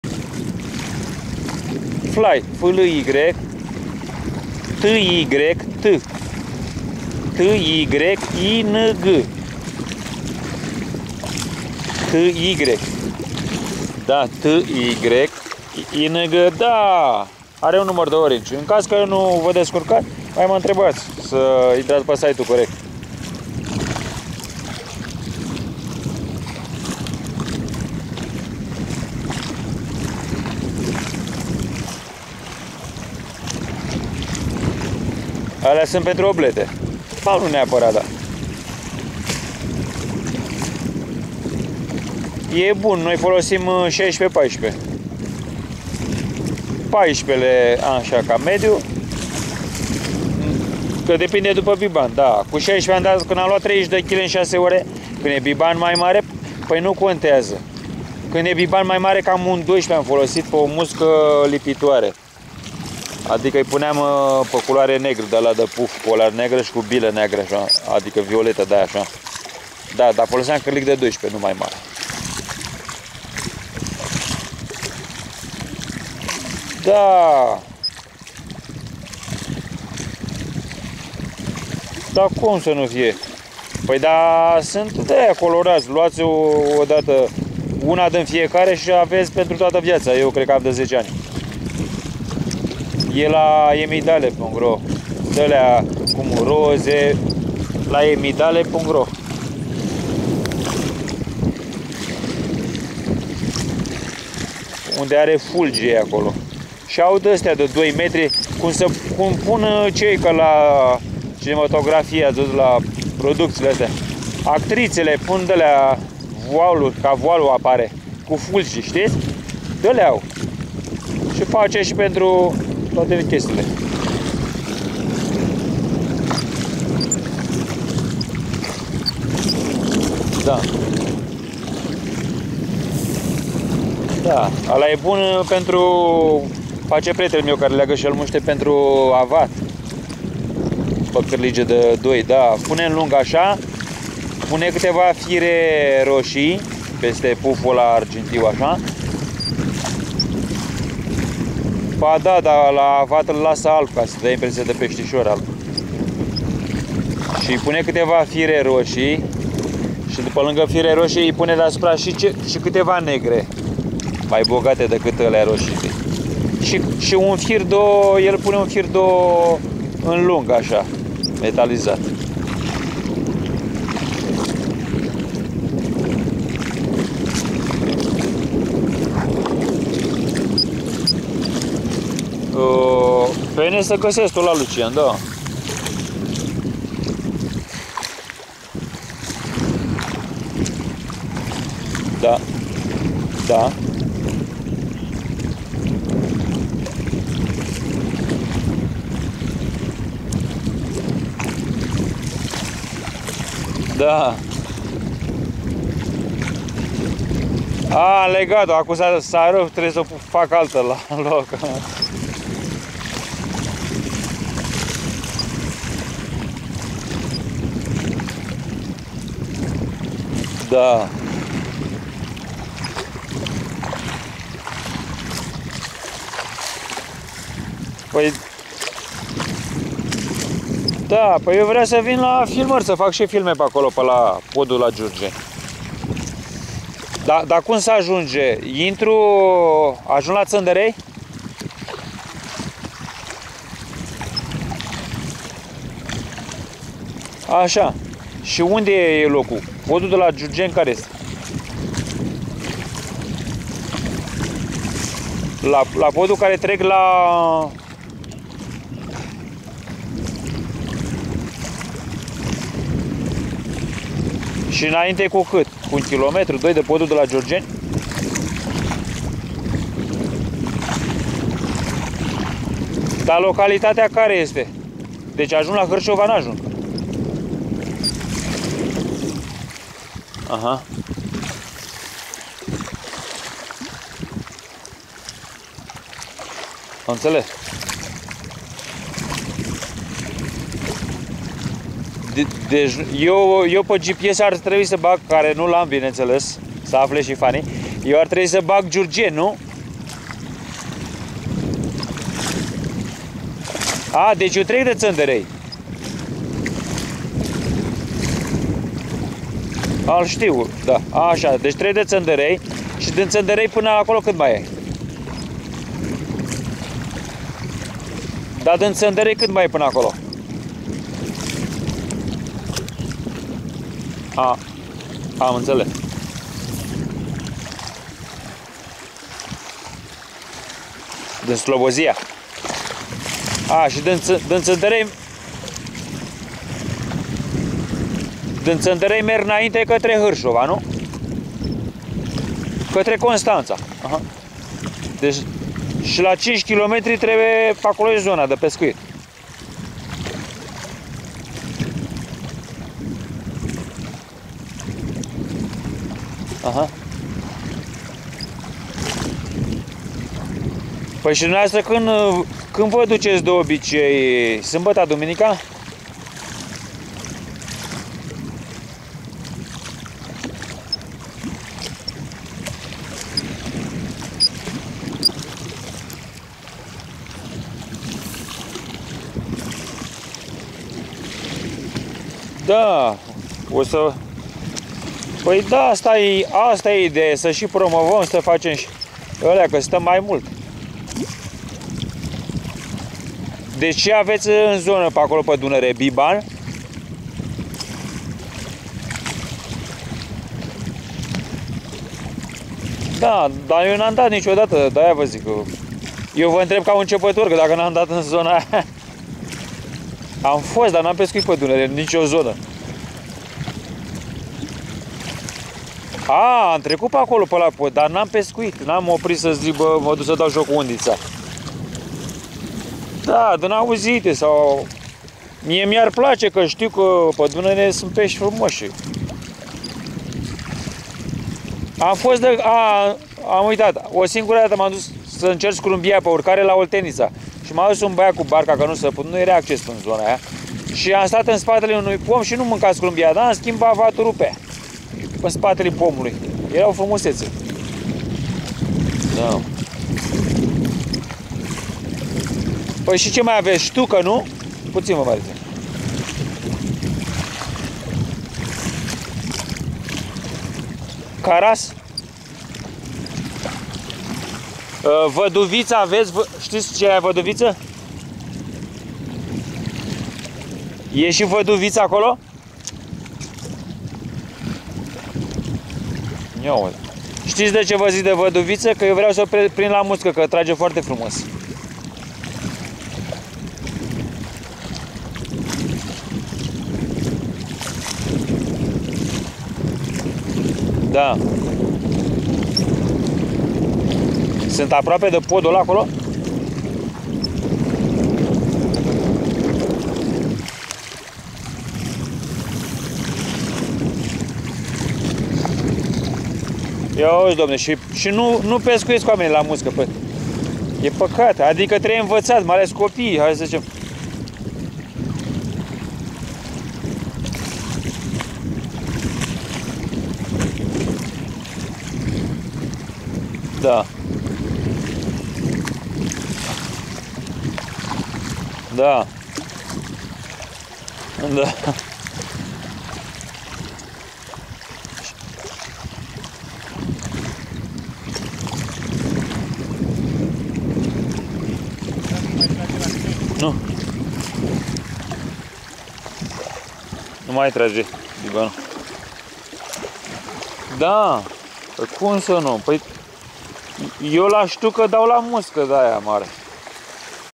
Pescuit bibani la muste artificiale baraj Udeni 6-08-2014
In acest film pescuiesc la muste artificiale la bibani la barajul Udeni si se rinde dar are toanele lui.Mai bine a mincat cind i-am atasat si bile fosforescente la mustele artificiale